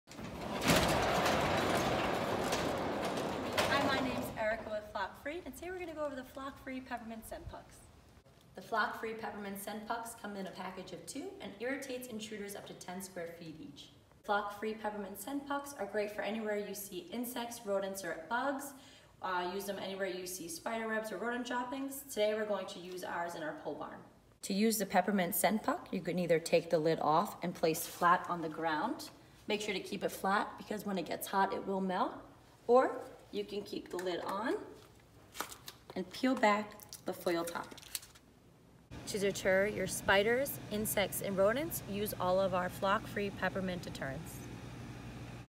How To Use Flock Free Peppermint Scent Pucks
This video shows you how to correctly use the Flock Free Peppermint Scent Pucks.